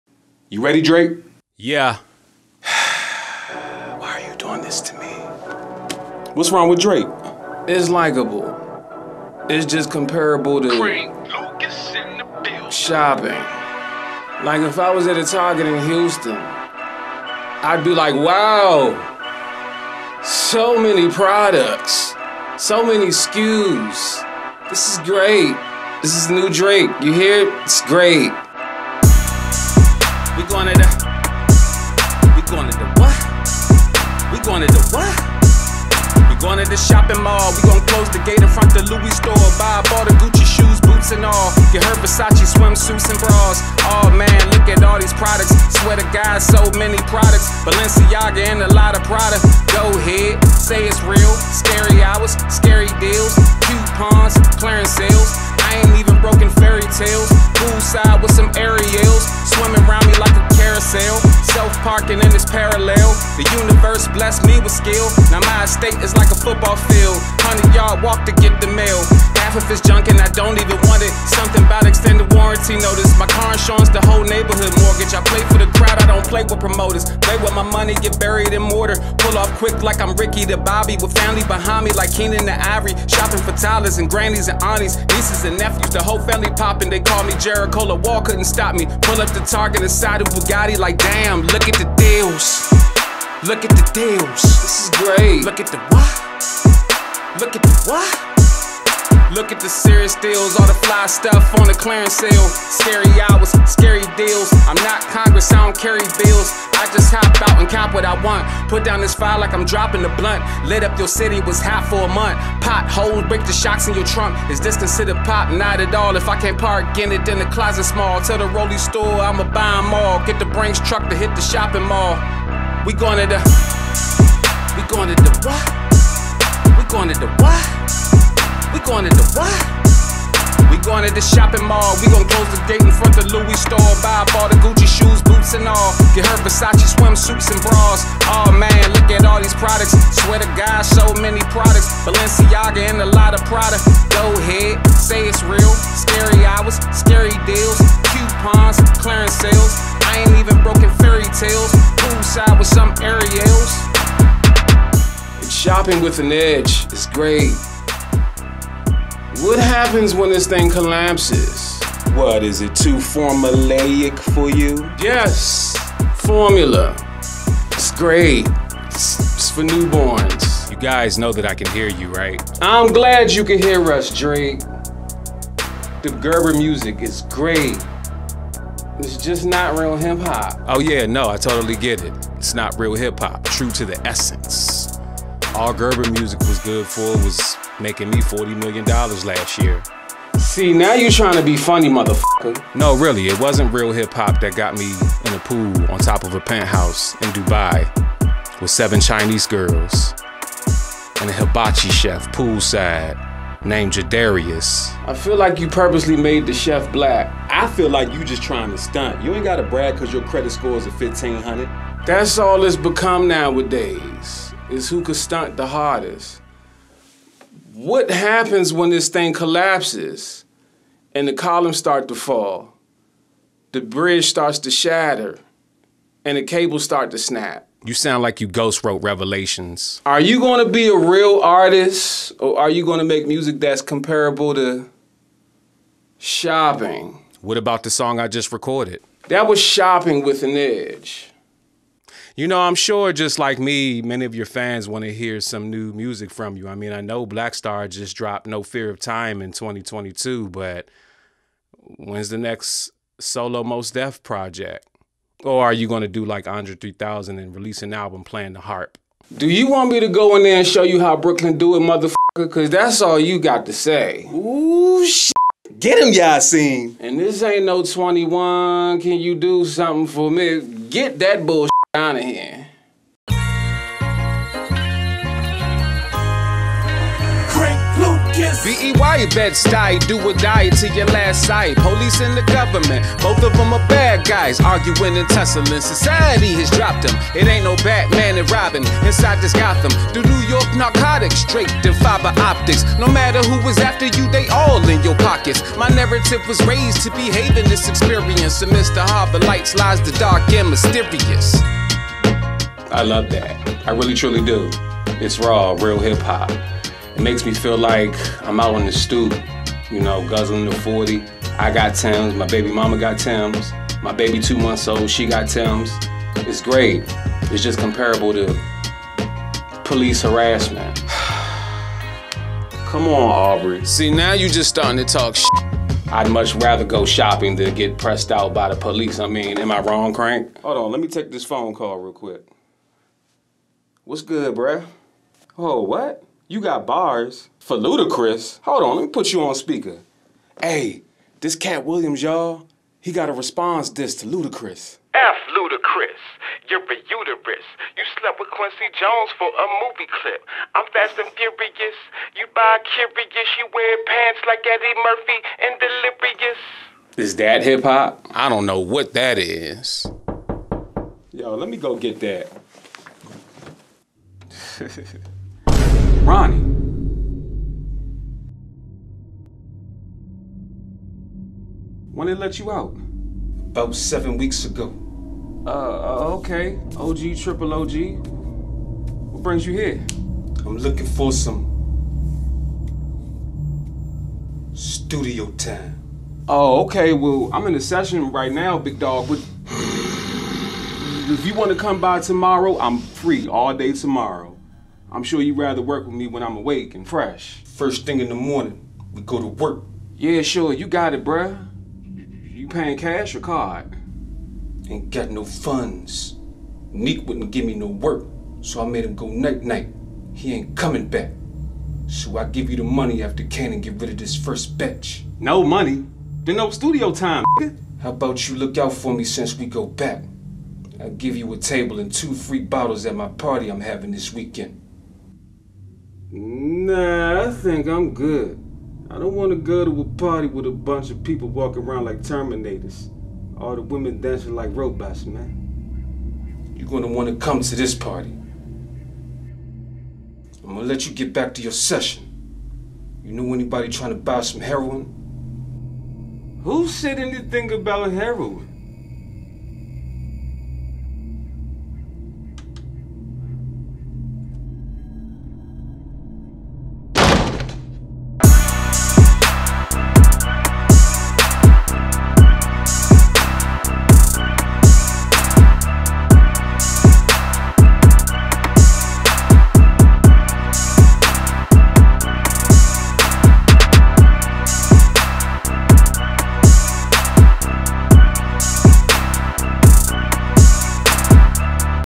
DRAKE AND MOS DEF IN THE STUDIO | Crank Lucas
After Mos Def commented about Drake being "pop" instead of "hip-hop" I got them together in the studio to work on some music. That was until Katt Williams called to share his Ludacris diss over the phone, and then I got a knock at the door.